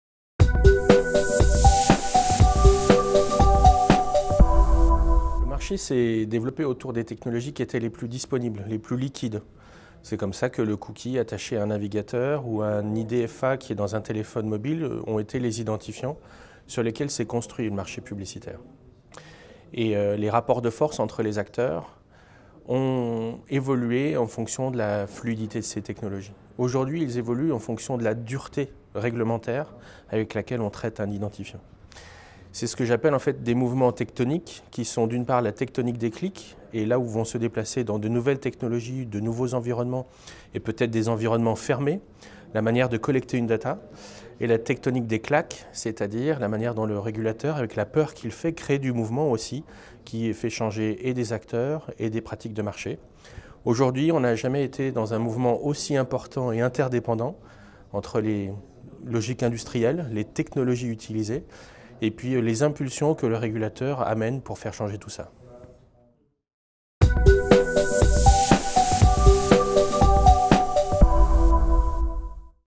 Quelle est l'évolution du marché et du RGPD ?
Interview de Etienne Drouard, Avocat Spécialisé Technologies de l'Information et Protection des données chez K&L Gates à l'occasion de la conférence "Mobile Marketing & RGPD : Comment transformer votre projet privacy en une opportunité ?"